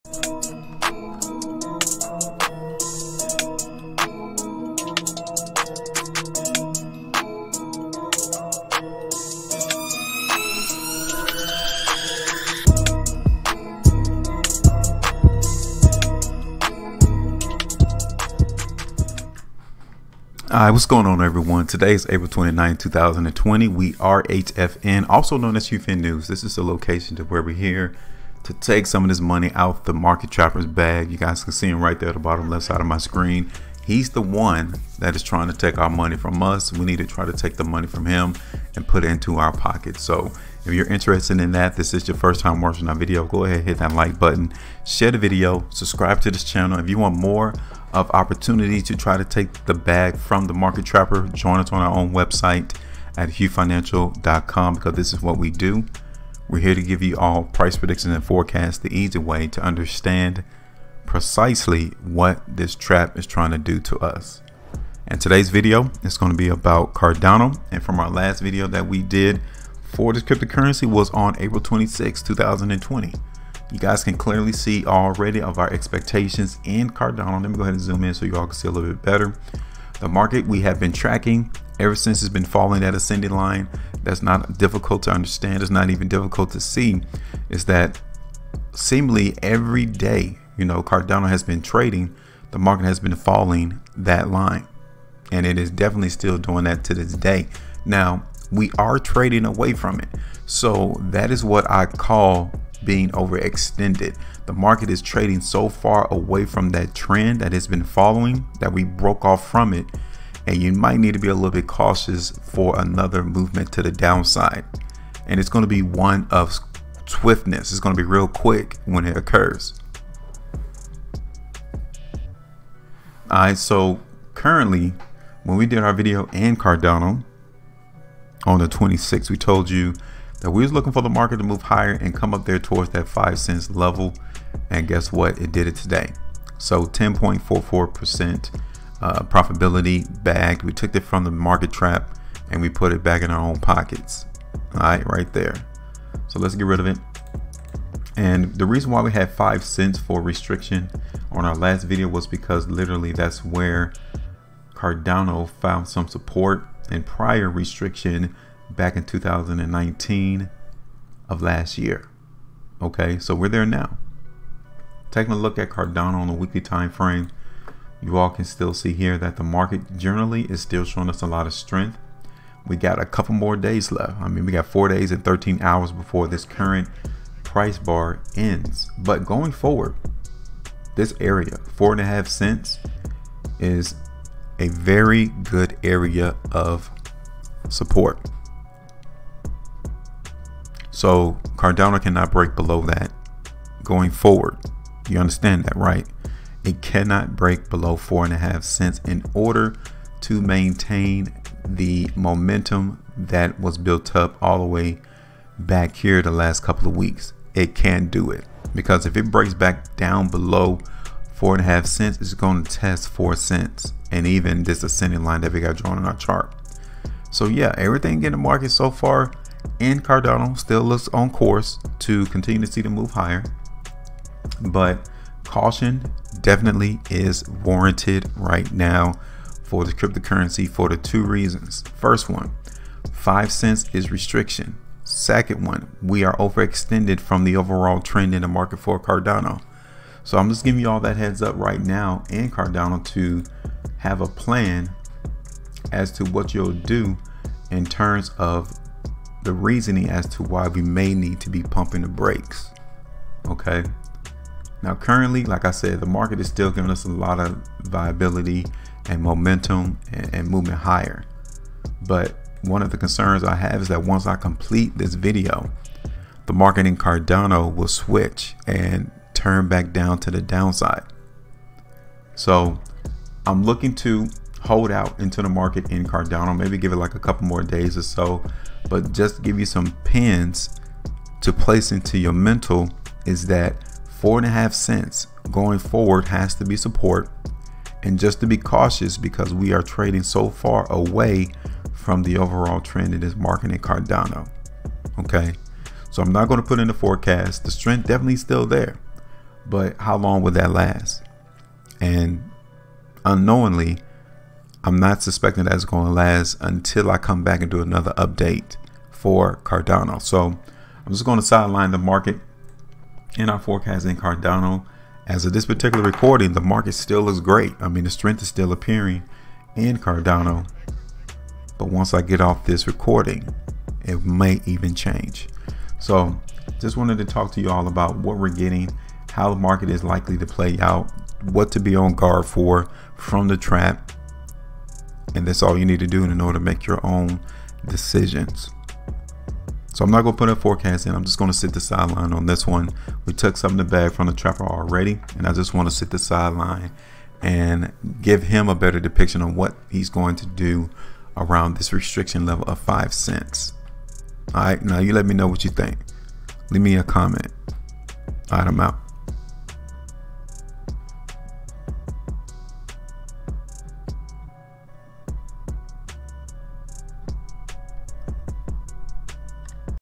Cardano (ADA) Price Update - Here's The Latest Information
Here is our latest Cardano (ADA) price update.  This is the latest information for Cardano (ADA).  We'll see if the price chart for Cardano shows continued strength and the prospect of movement higher.  Let's see.